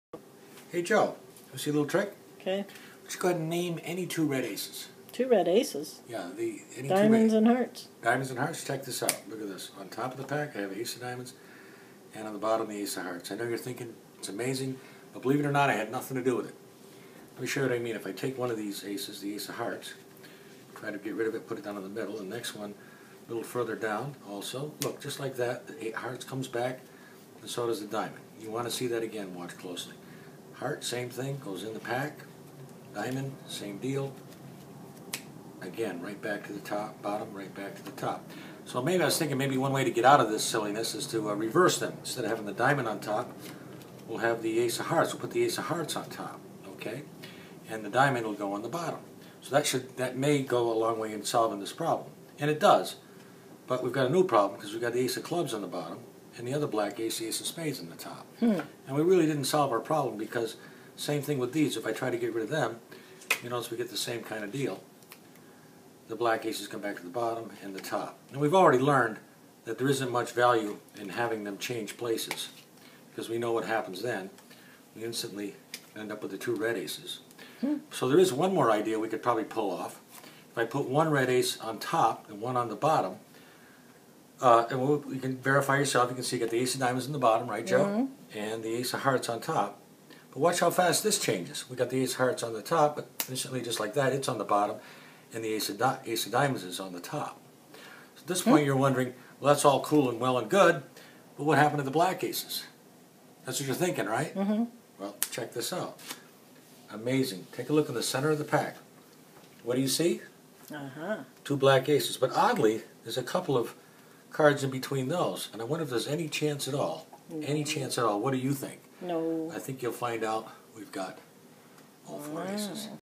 Bert Allerton's "Pump Room Phantasy" Revisited
The original routine, from the "Stars of Magic" series, almost 70 years old, is still a wonderful routine as it is, a perfect worker where there are no angle issues, and there is not a single "hidden" move. I've tried to keep the character and charm of the original while bringing a couple more visual moments to it. I've maintained the "No angle issues" property, but have added a couple very simple sleights that still do not add to the difficulty or complexity.